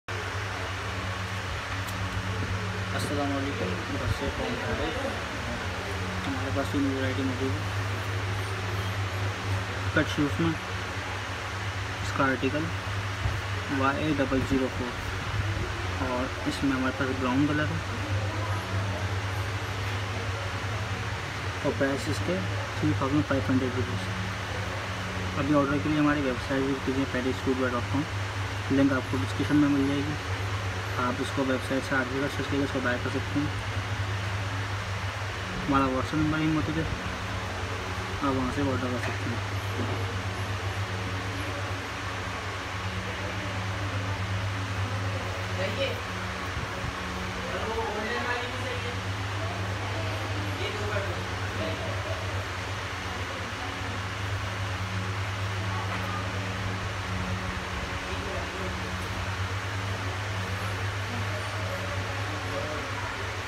YA004-SHORT REVIEW BY PARISH FOOTWEAR
Salam parish is here with short review of : YA004
Visit Our Outlet : Shop G-1, Dolmen Mall ,Tariq Road Karachi.